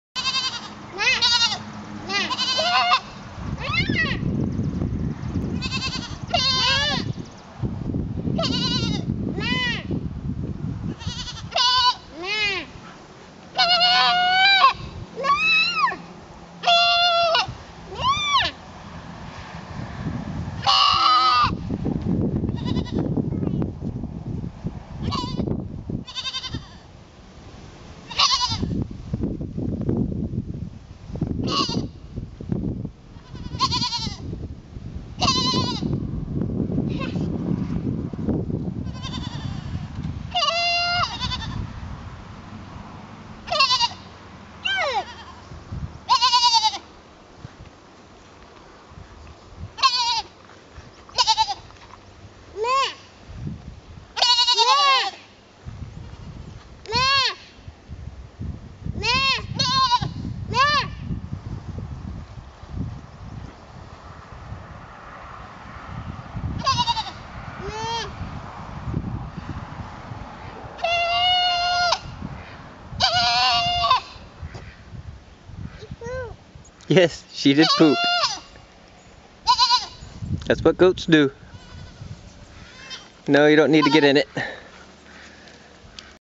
Piper being a goat
Piper was trying to fit in the herd.